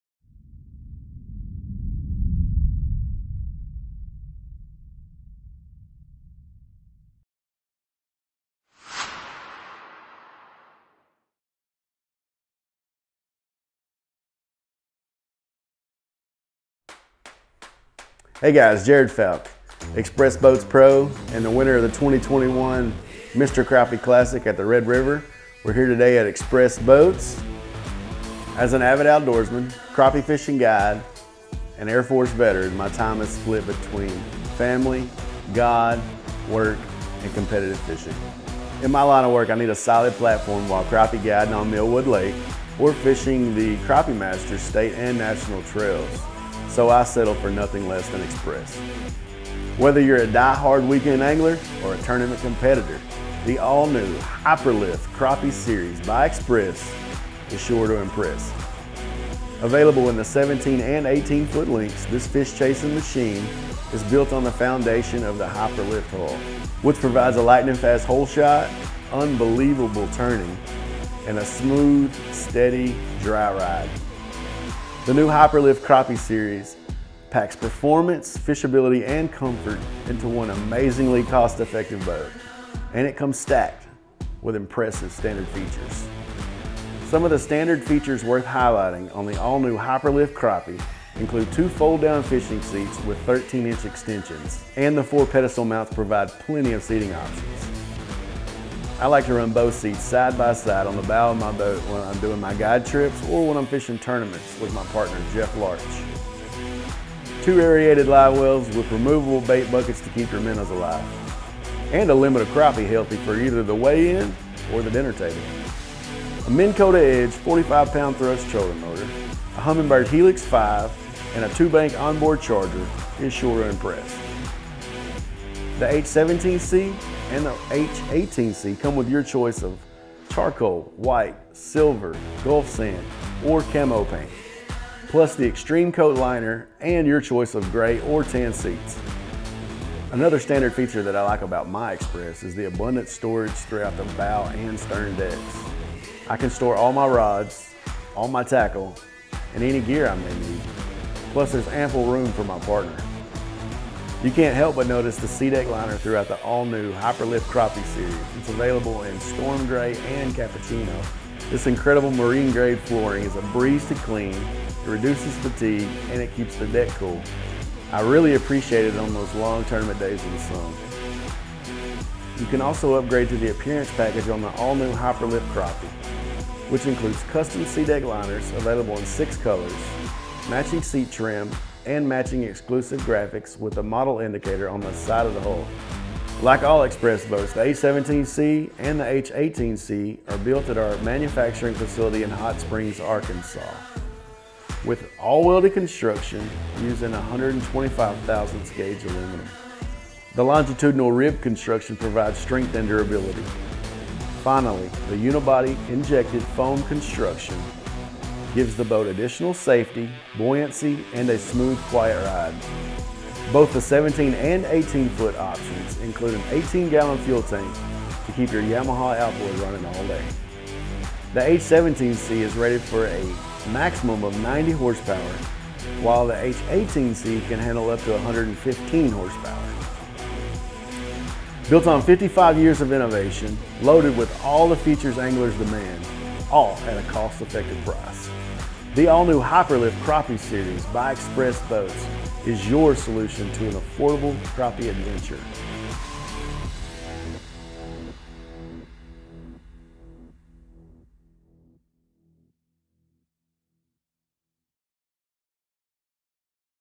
2022 H17C H18C Crappie Overview
The Xpress Boats all-new 2022 Hyper-Lift Crappie Series is built on top of the original, aluminum padded hull design, years of innovation, and the essential features anglers demand, all at a cost-effective price. Available in 17 and 18-foot lengths, this fish chasing machine has an ergonomically designed interior and is stacked with impressive standard features. This fast, dry hull design is your solution to getting to the brush pile and back quickly and safely. Whether competing in a tournament or a weekend angler, the new Hyper-Lift Crappie Series by Xpress is sure to impress!